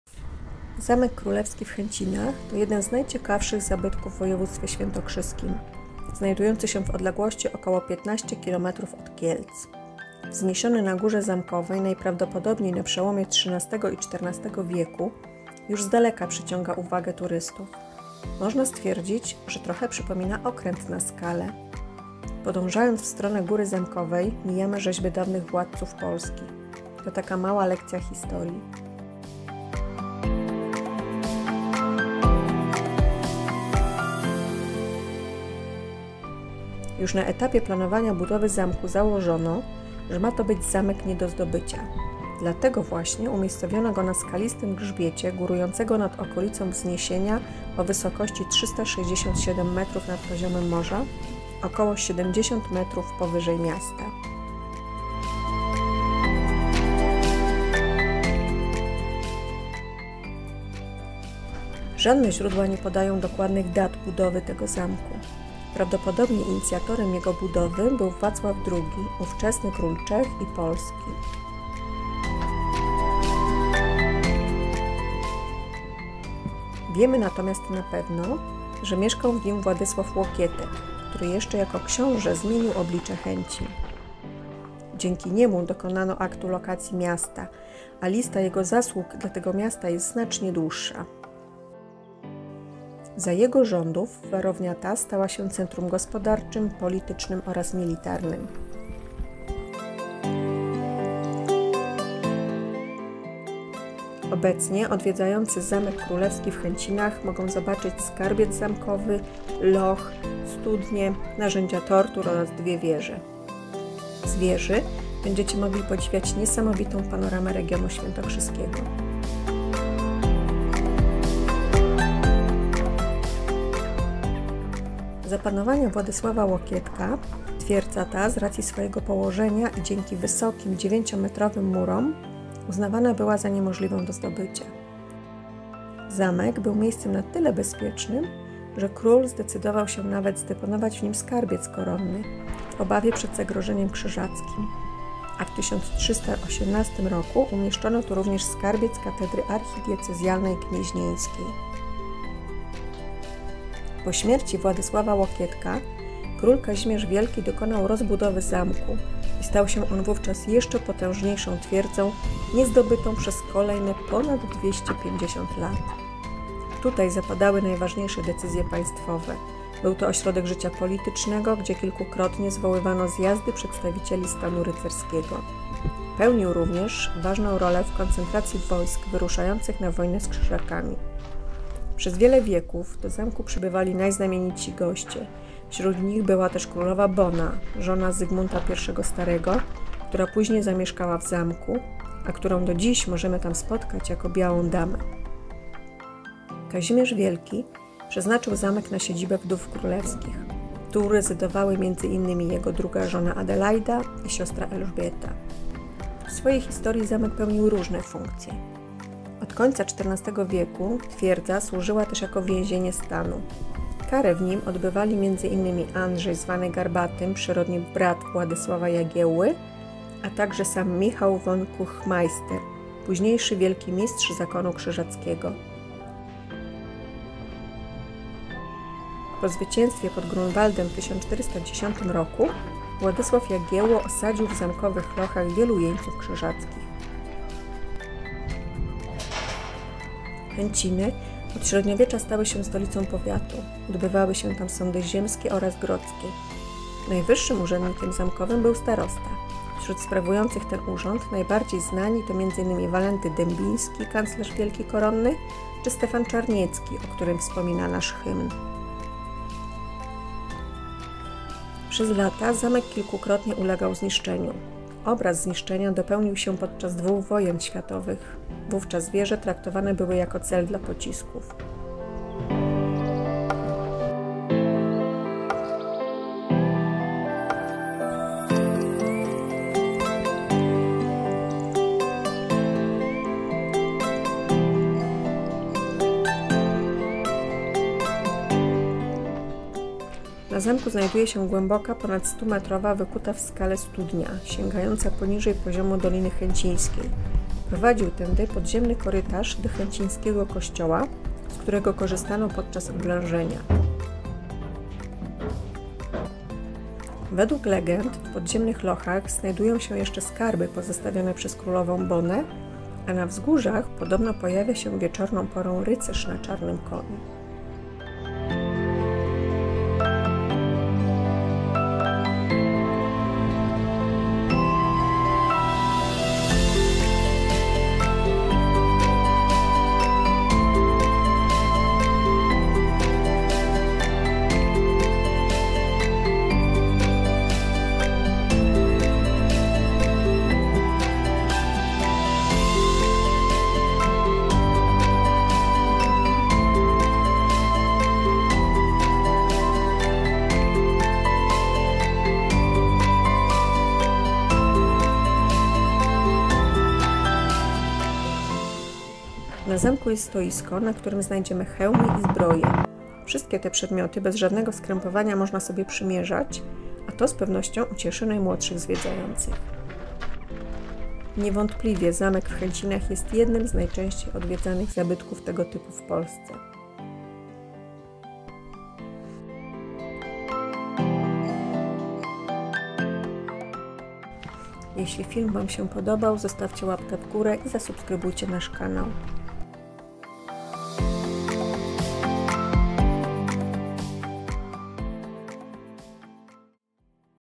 Zamek Królewski w Chęcinach - Zamki w Polsce - woj. świętokrzyskie
Tym razem zwiedzamy ruiny Zamku Królewskiego w Chęcinach w woj. świętokrzyskim około 15 kilometrów od Kielc. Zamek średniowieczny położony na wzgórzu, wyglądający jak okręt umieszczony na skale.